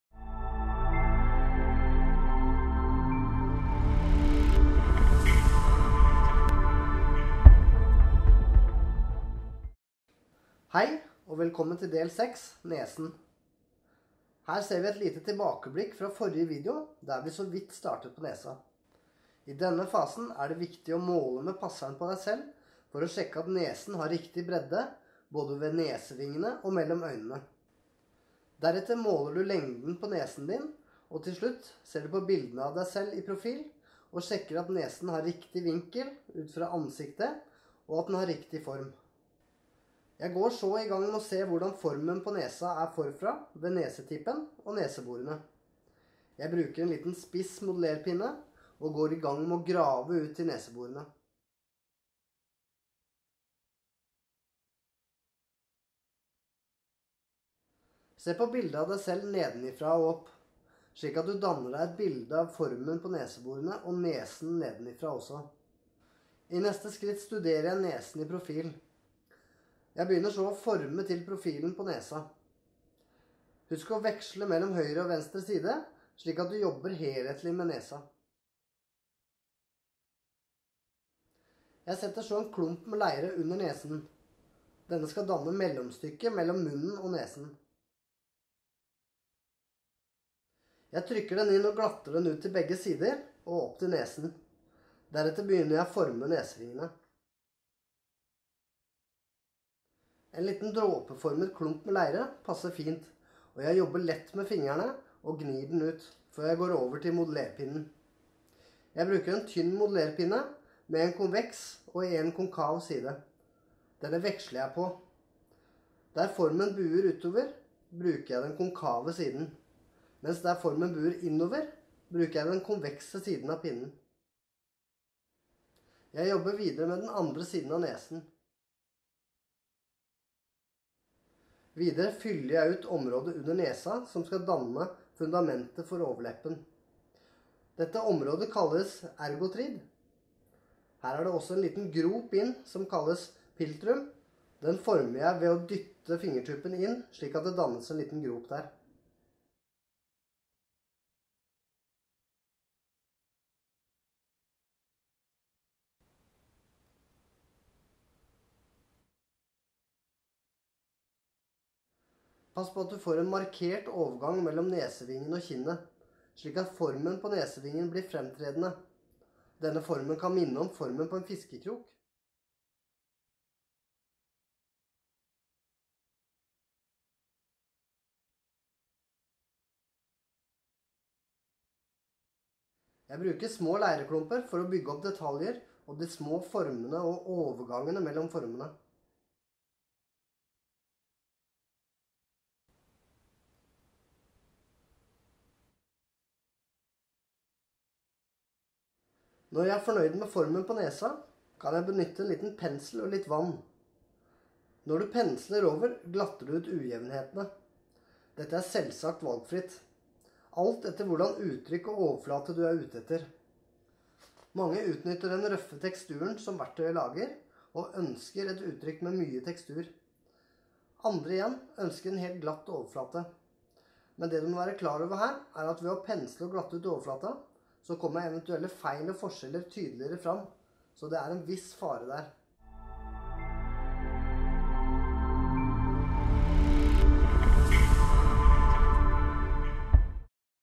Del 6 Nesen
Videoen viser hvordan man kan gå frem for å forme nesen i et selvportrett.
Bildene i videoen er ment for å gi eksempler på hvordan forskjellige skulptører har fremstilt nesen og ikke minst inspirere.

For mer informasjon om bildene kan du sjekke ut lenkene under.

1. Selvportrett eget foto

2. Pierre Auguste Renoir "Venus Victrix" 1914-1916 (Eget foto)

5:Selvportrett eget foto

6:Christophe Charbonell "Tete de pilote" 2009

7:"Fiskekrok" Egen illustrasjon

8: Xavier Veilhan "Alice" 2013

9:Xavier Veilhan "Alice" 2013